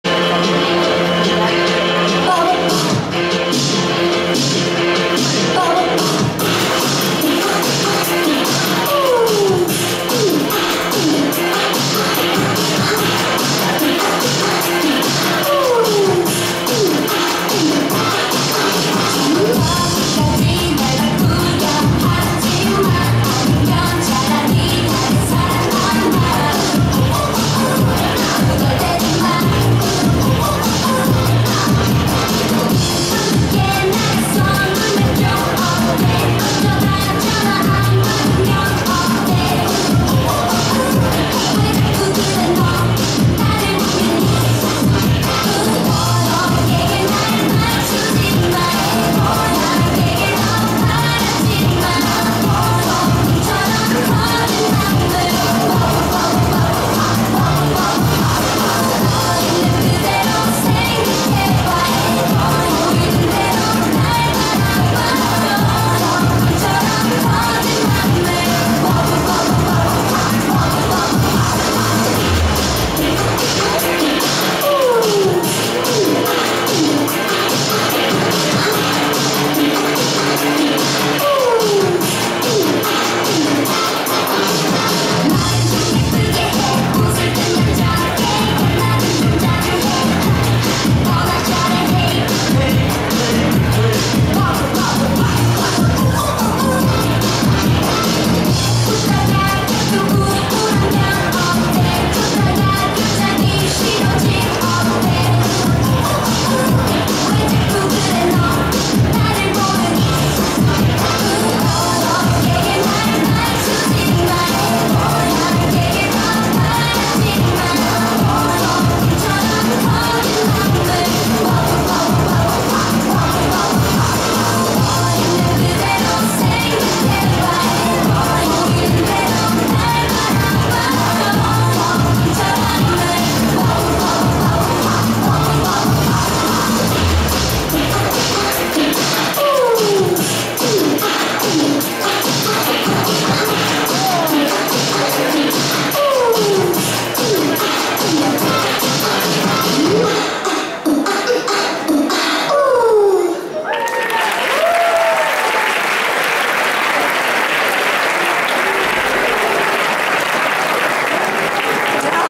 [EsCosplay] Expomanga 2015 - Death Note
Actuación del concurso de cosplay de Expomanga 2015 con cosplay de Misa de Death Note